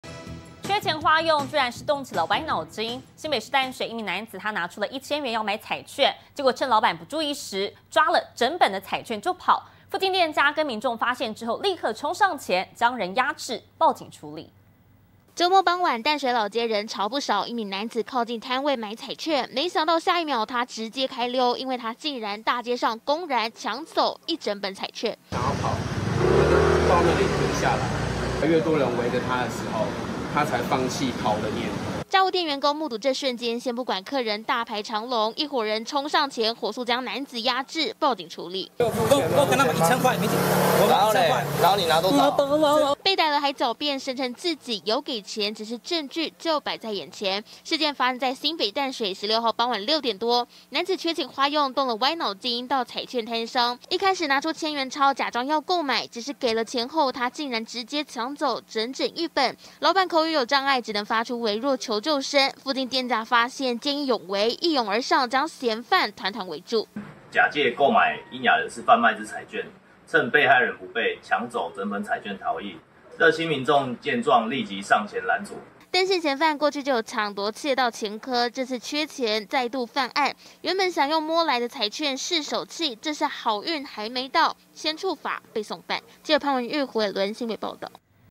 拿千元假意購買 男搶喑啞老闆整本彩券落跑
缺錢花用，竟在大街上直接搶走彩券攤整本彩券!男子在 新北淡水，拿出一千元買彩券，沒想到他趁老闆不注意，直街搶了桌上整本彩券就跑，附近店家與民眾發現，老闆發出微弱的求救聲，大家立刻衝上去將人包圍壓制、報警處理。